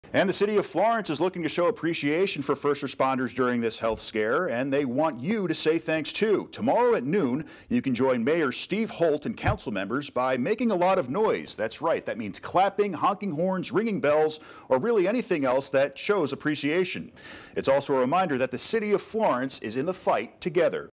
Make noise to thank Florence first responders
Mayor Steve Holt wants noise, lots of noise, on Wednesday April 8 at noon to celebrate Florence first responders.